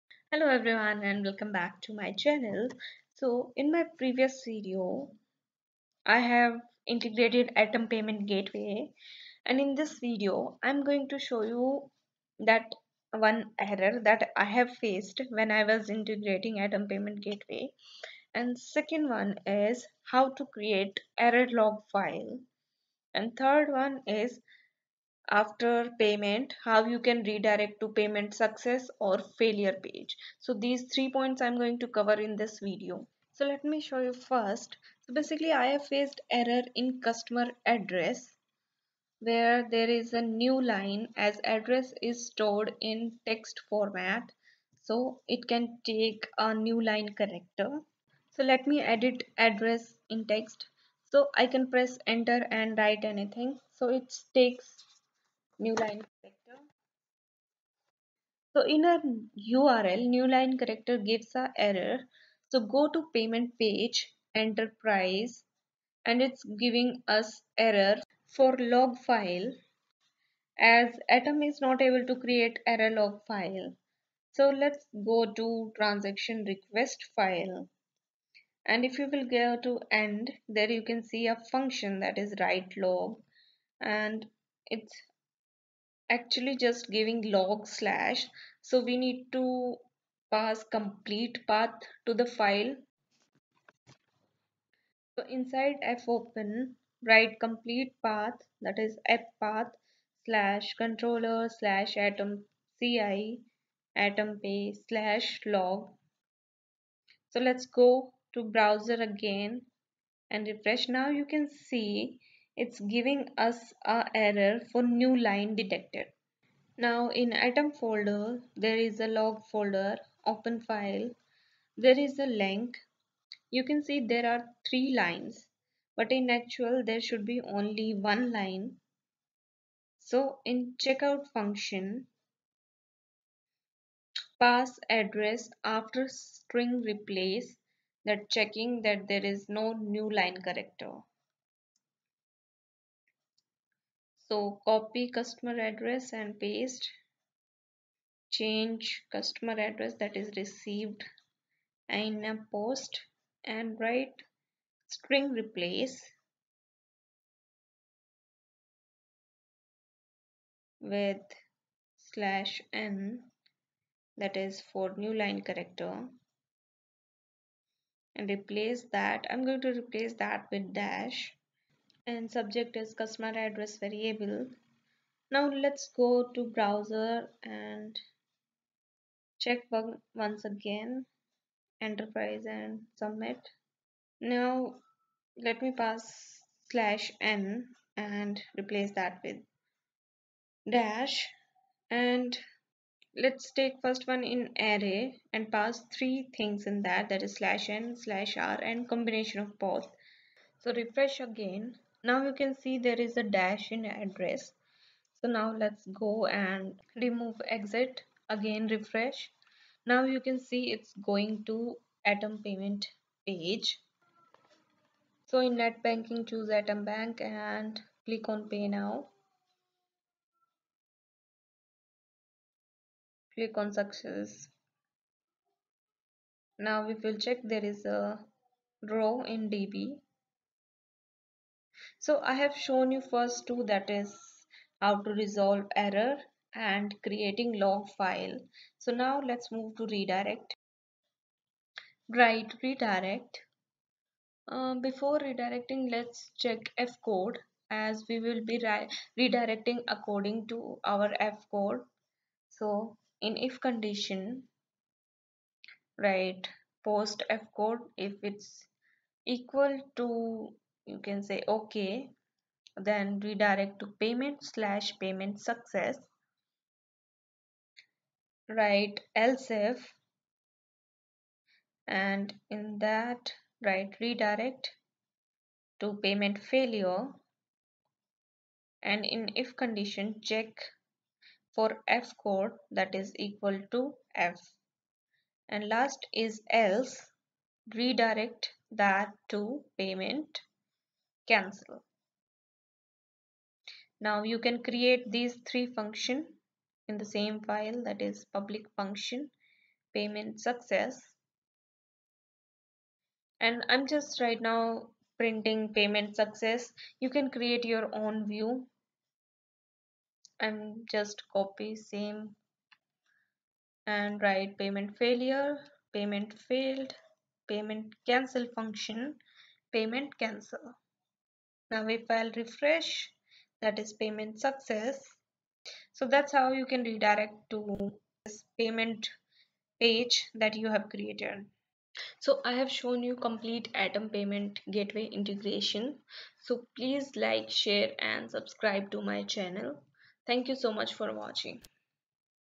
How to Integrate Atom Payment Gateway Integration in Codeigniter | Store Response in Database |Part2
In this video, you will learn how to integrate atom payment gateway in codeigniter.

Steps:
1. Create a controller for payment
2. Include atom payment files into controller
3. From sample file , copy the function set and paste it into your controller function (checkout).
4. Pass all the required fields.
5. Transaction password need to be send in password
other password is used for login in merchant panel.
6. Redirect to atom link
7. After that, create a return url and create same function in your controller
8. In return url function, you will be getting transaction response.
9. In response you will be getting signature and a validation function will be called to validate your response.
10. All the payment response fields, store it in a database.

How to Integrate Atom Payment Gateway Integration in Codeigniter | PHP | Store Response in Database